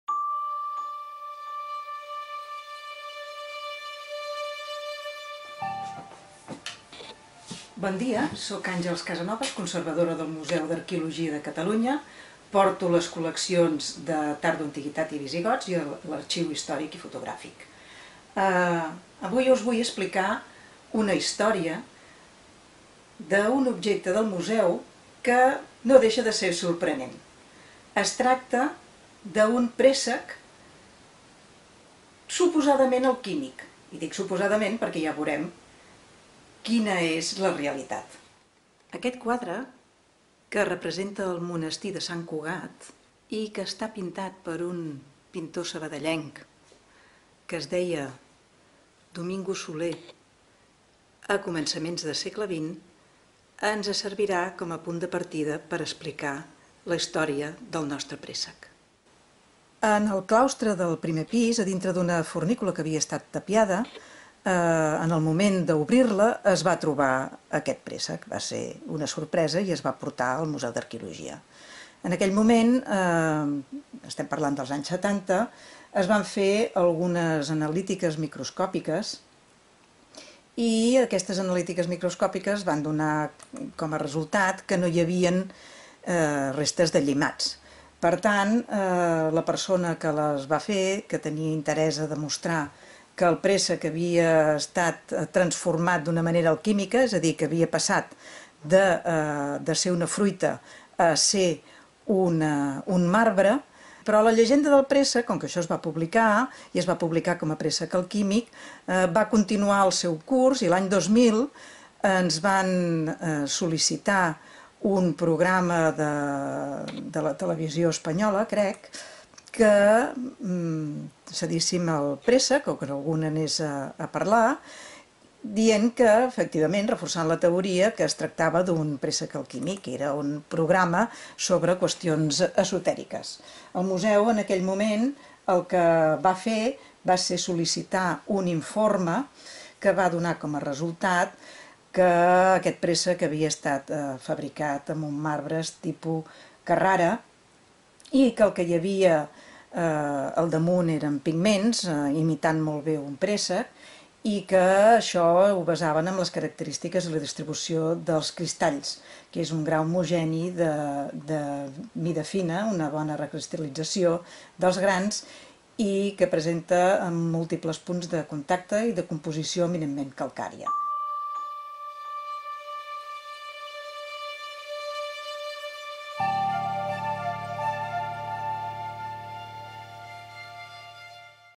MuseuEndins - El misteri del préssec alquímic, amb Àngels Casanovas
A principis dels anys 70 va aparèixer un préssec dins d'una fornícula tapiada del claustre del monestir de Sant Cugat. La conservadora del MAC, Àngels Casanovas, ens explica el misteri d'aquest curiós objecte en una nova entrega de MuseuEndins.